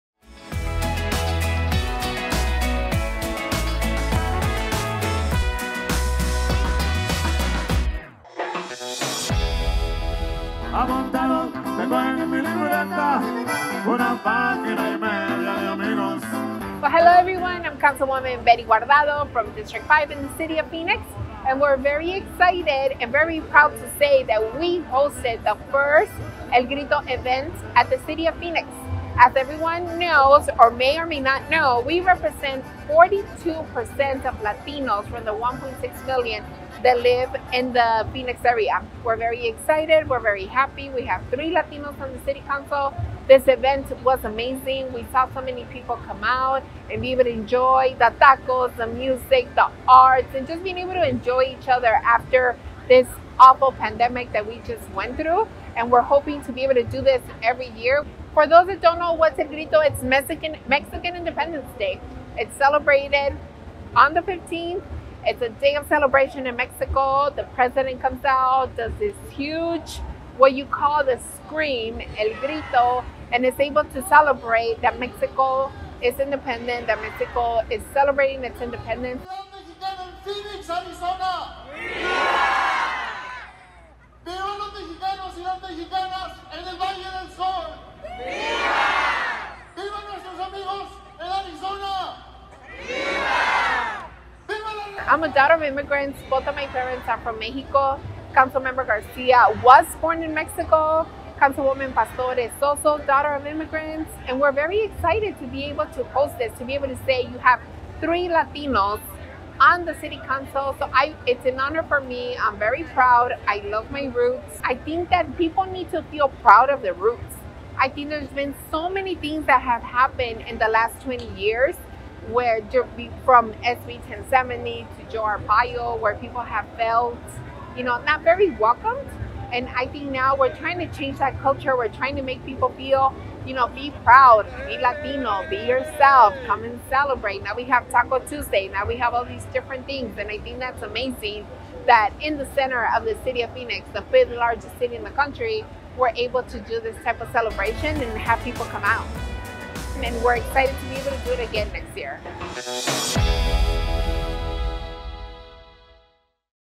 Councilwoman Guardado Hosts El Grito Celebration to Kick off Hispanic Heritage Month
Councilwoman Betty Guardado, Councilmember Carlos Garcia, and City of Phoenix Sister Cities Commission kicked off Hispanic Heritage Month with an El Grito celebration in Downtown Phoenix. The special tradition takes place every year on the night of September 15, and celebrates Mexican Independence Day. This year’s event downtown featured live music, dancing, arts & crafts, ballet folkloric, food, and more.

Latest News: Phoenix.gov/Newsroom
Web: Phoenix.gov
Social: @CityofPhoenixAZ on Facebook, Twitter and Instagram